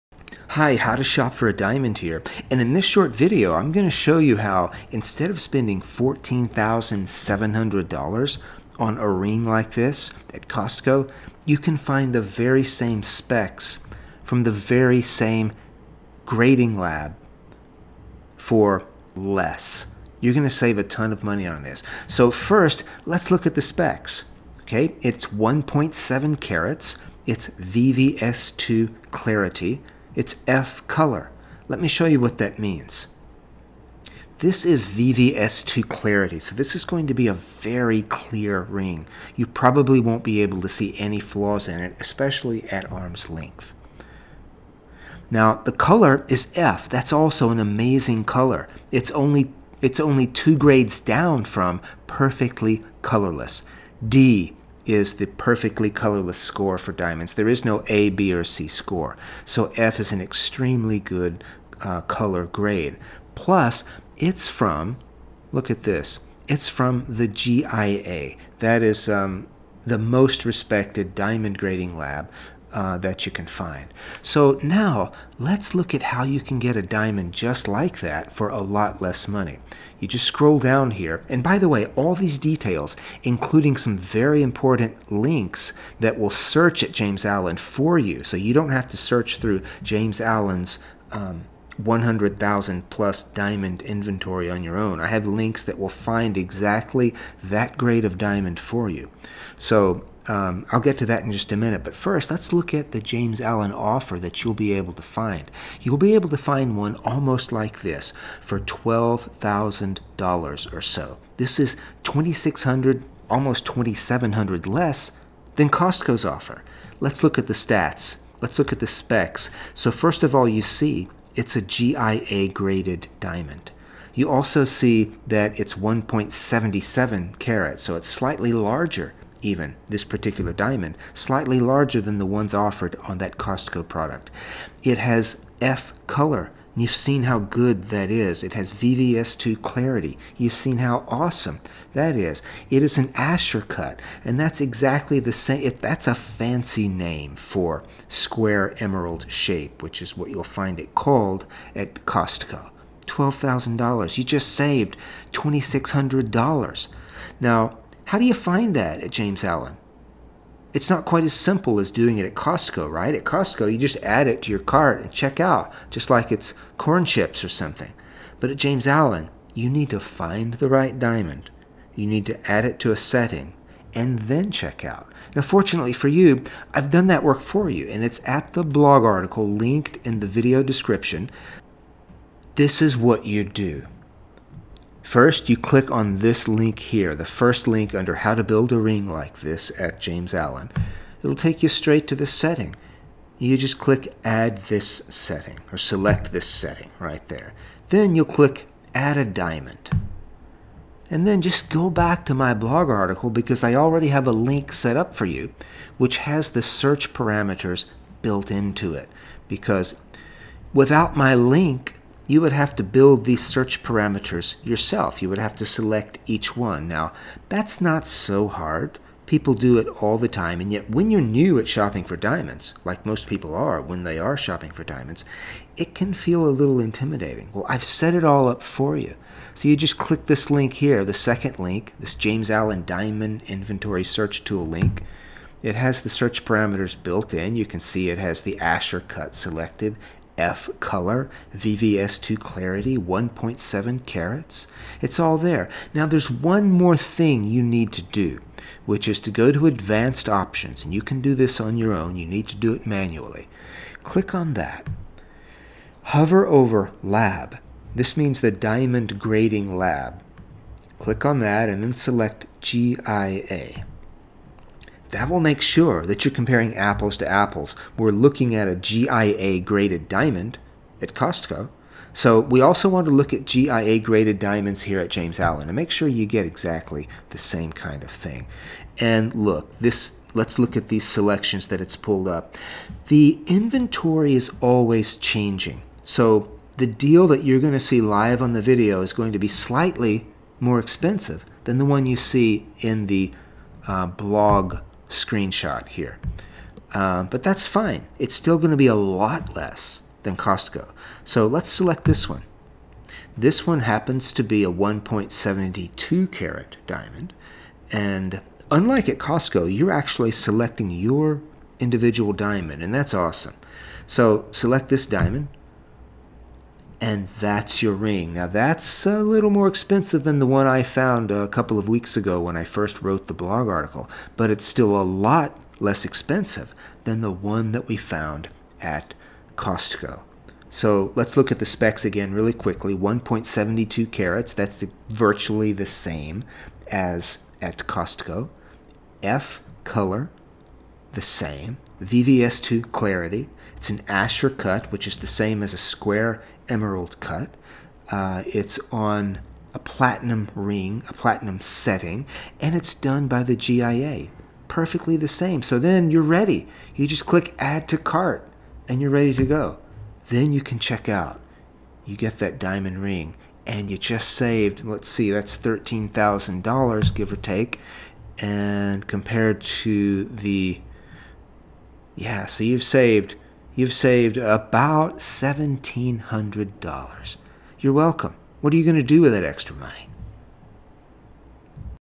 Costco 1.7C Diamond Ring for $15K vs $13K at James Allen
You can save $1,300 to $2,500 or so with the links and search settings I show you in this video. Go to the article for full proof and the search